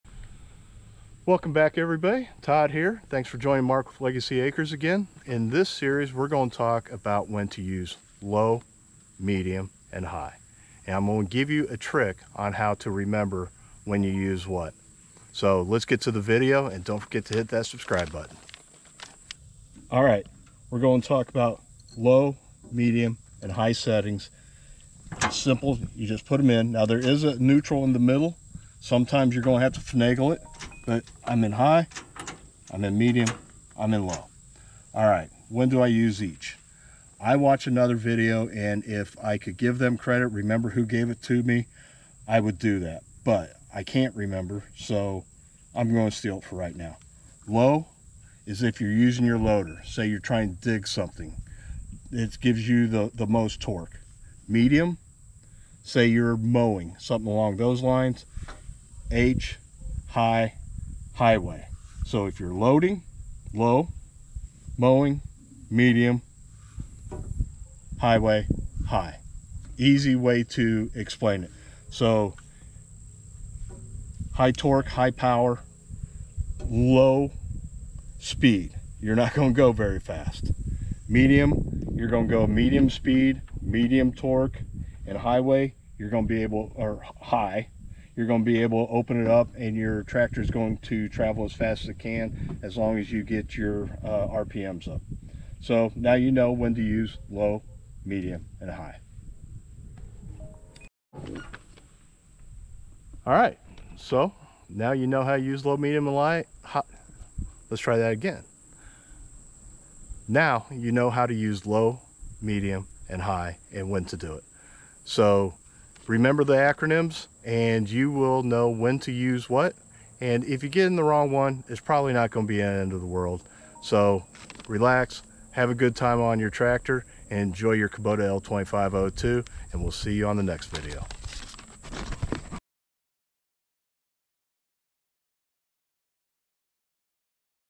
Kubota L2502 - High / Medium / Low Gear Settings "Made Easy Series" Ep. 6
Learn an acronym of when to use High, Medium or Low gear settings.